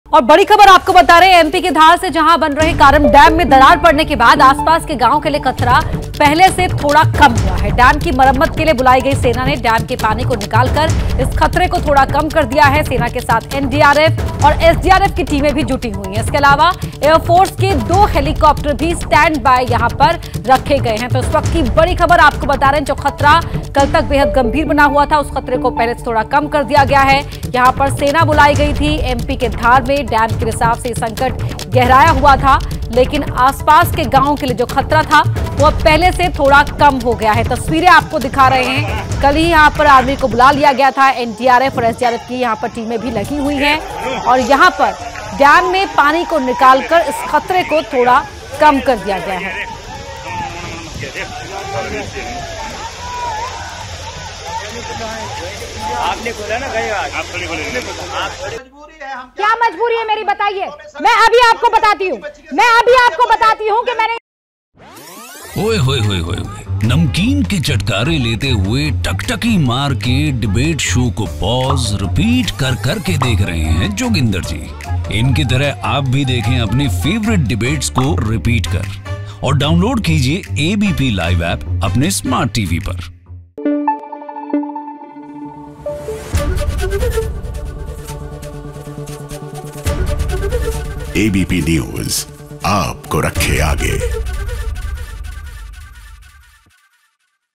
Breaking News : MP के धार में डैम के रिसाव से संकट गहराया | Madhya Pradesh News | ABPLIVE
मध्य प्रदेश (Madhya Pradesh) के धार खरगोन क्षेत्र के बीच कारम नदी (Karam River) पर बने डैम में दरार आ जाने के कारण आसपास के ग्रामीण क्षेत्रों में बीती रात दहशत फैल गई. प्रशासन ने बाढ़ की आशंका को देखते हुए और किसी प्रकार की अनहोनी को रोकने के लिए आसपास के कई गांव को खाली करा लिया गया है.रात के समय में ही प्रशासन ने मुस्तैदी से मुनादी कर लोगों को सूचित किया था.बांध को किसी प्रकार के नुकसान से बचाने के लिए सेना (Army) को बुलाया गया है. सेना वहां पहुंच गई है. इसके साथ ही एनडीआरएफ और एसडीआरएफ की टीमें भी तैनात की गई हैं.